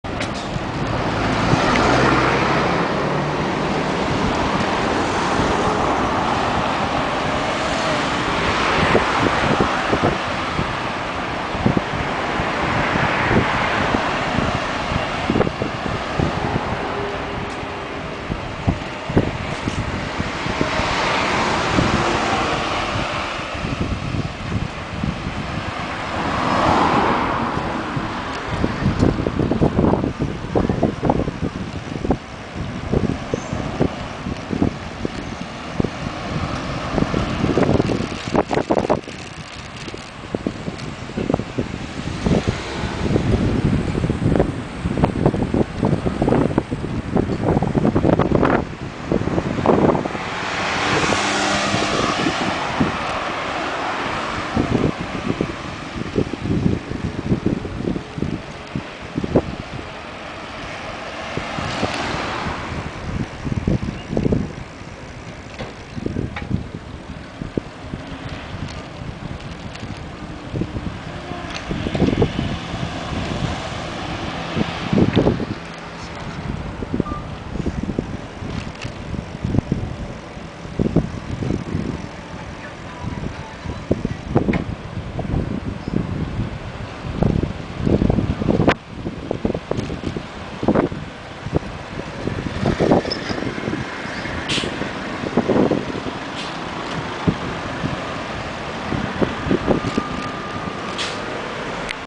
PSPを自転車に装着
PSPナビを活用するべく、PSPを自転車に装着して走行してみました。